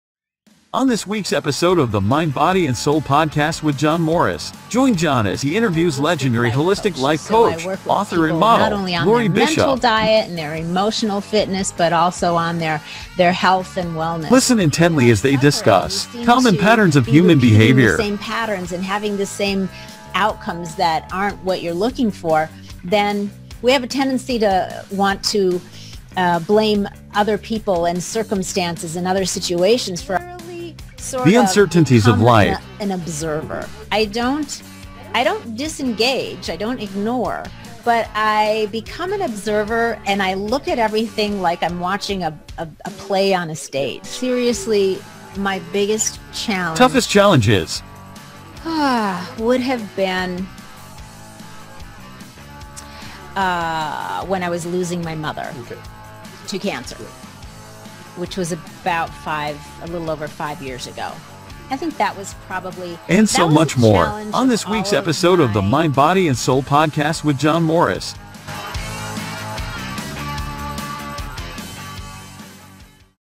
MBS| Episode 2 | Loree Bischoff Teaser | John Morris
In this weeks episode of the Mind, Body and soul podcast with John Morris we will be joined by Model, author and holistic life coach Loree Bischoff. Who is also the wife of the WWE legend Eric Bischoff.
In this episode we will be talking about:
- Coping after losing a parent to cancer
- Toughest battles
- The life of an Entrepreneur and so much more...

Join the Mind, Body & Soul adventure.

to read, hear, and watch great teaching.